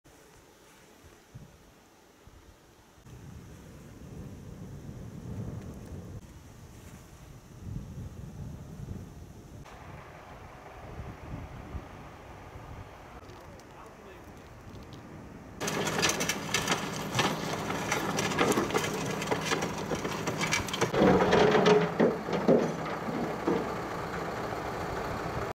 Automobiliste gewond na botsing met boom in Klijndijk
KLIJNDIJK -  Een automobiliste is maandagavond naar het ziekenhuis gebracht nadat hij met zijn voertuig tegen een boom was gereden op de Slenerweg. Over de oorzaak is nog niets bekend. De politie doet onderzoek en legt de sporen vast. De auto was dusdanig beschadigd dat een berger het voertuig moest afslepen. De Slenerweg werd afgesloten voor het verkeer.